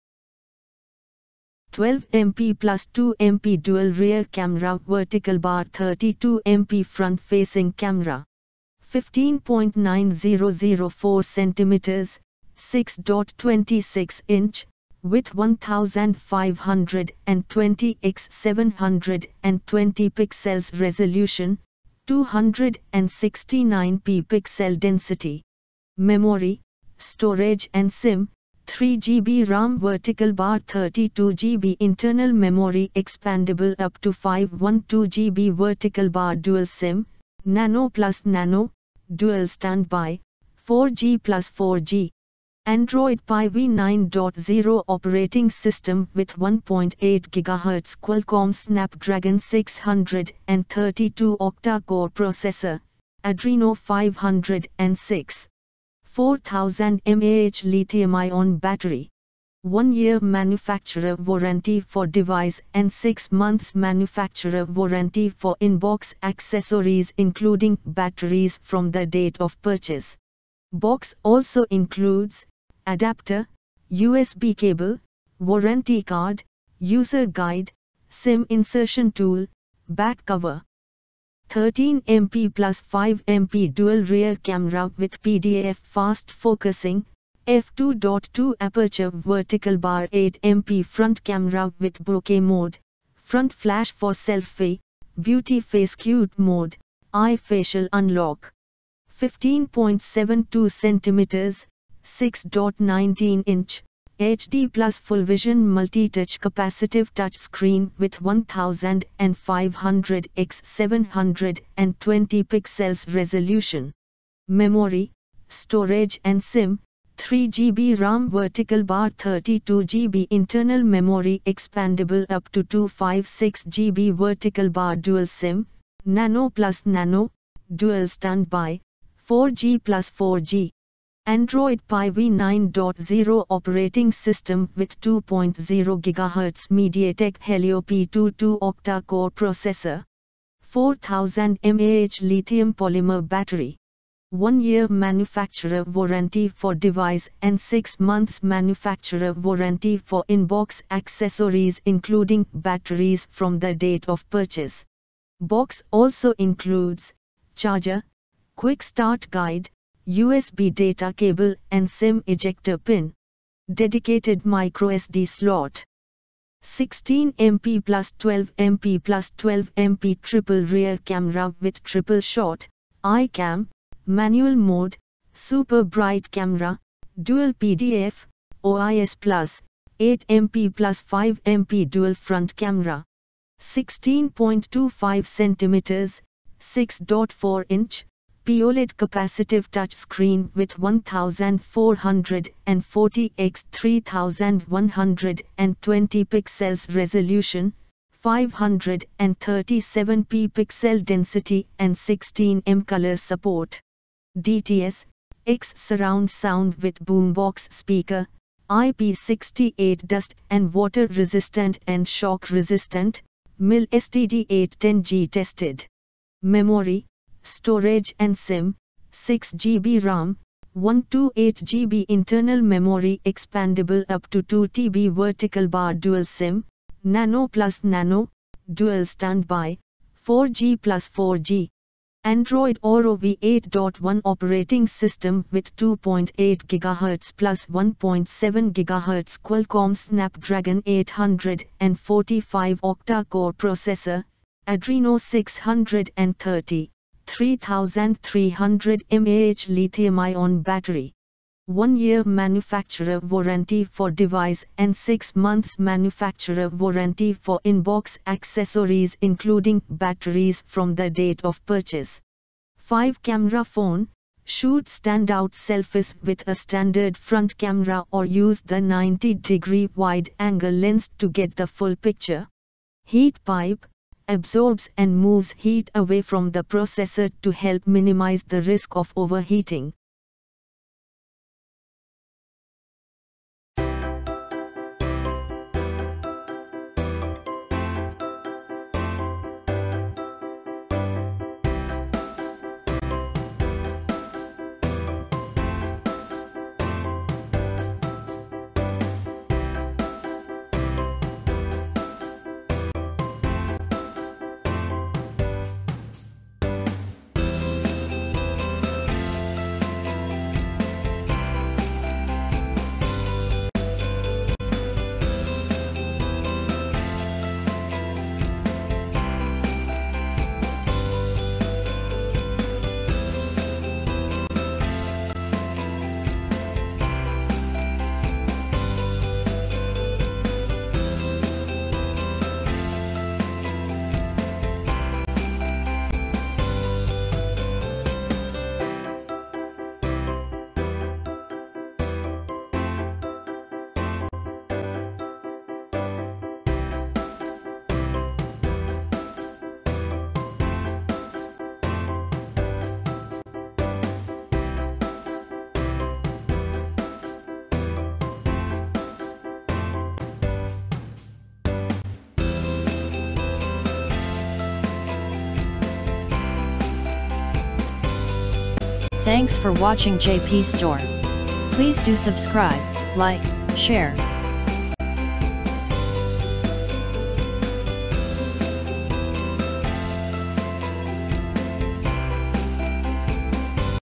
Redmi Y3 (Elegant Blue, 3GB RAM, 32GB Storage)
12MP+2MP dual rear camera | 32MP front facing camera
15.9004 centimeters (6.26-inch) with 1520 x 720 pixels resolution, 269 ppi pixel density
Memory, Storage and SIM: 3GB RAM | 32GB internal memory expandable up to 512GB | Dual SIM (nano+nano) dual-standby (4G+4G)
Android Pie v9.0 operating system with 1.8GHz Qualcomm Snapdragon 632 octa core processor, Adreno 506
4000mAH lithium-ion battery
1 year manufacturer warranty for device and 6 months manufacturer warranty for in-box accessories including batteries from the date of purchase
Box also includes: Adapter, USB cable, warranty card, user guide, SIM insertion tool, back cover